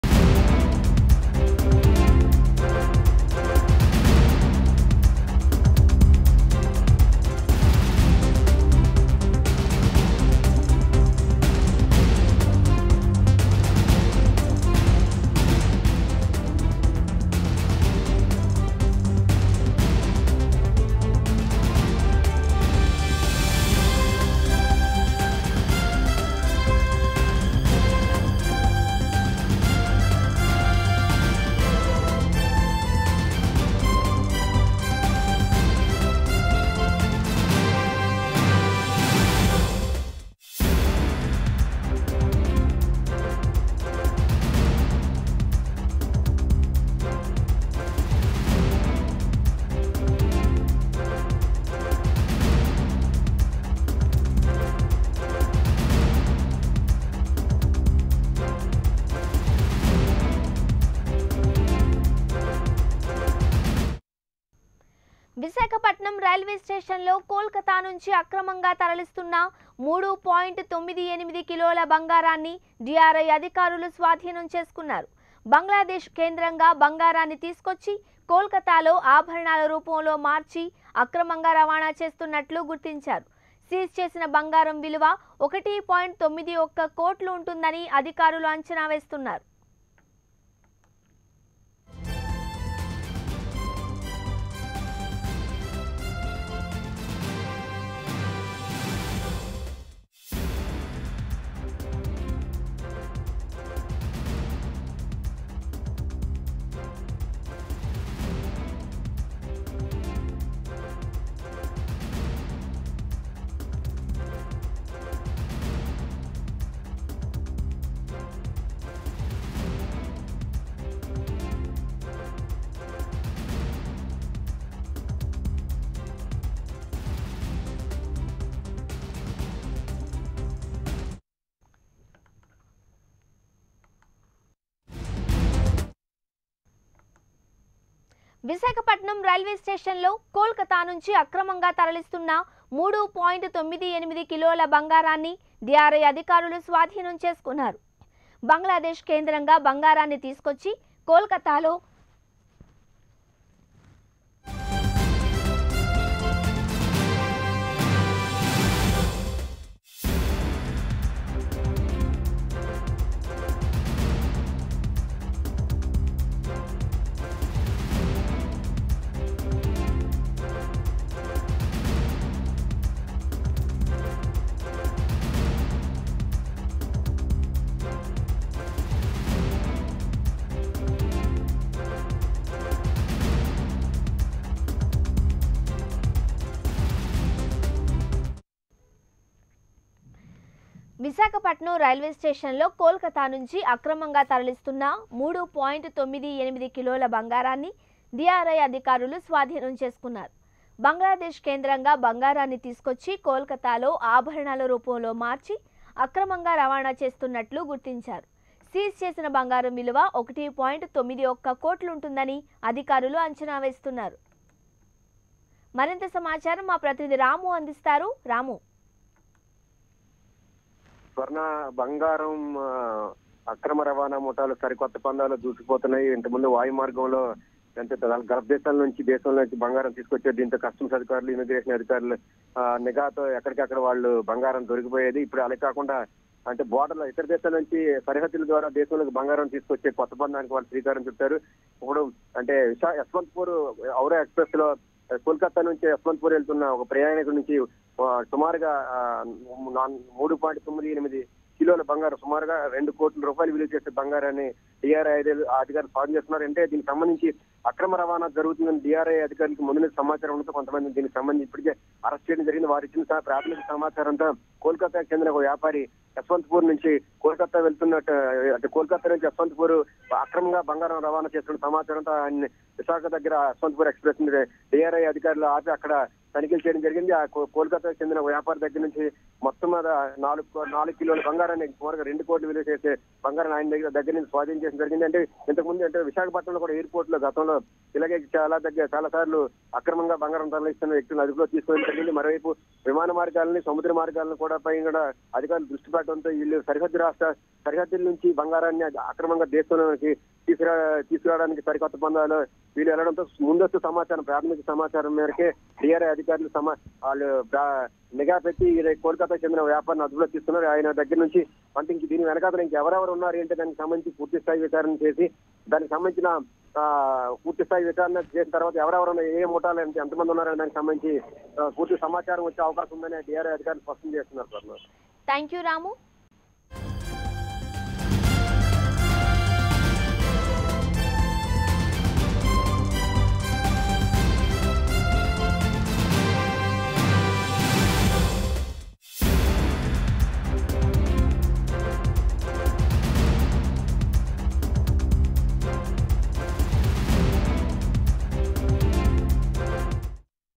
అక్రమంగా తరలిస్తున్న దాదాపు 4కేజీల బంగారం పట్టివేత | Illegal Gold in Howrah Express | Prime9 News
Illegal Gold in Howrah Express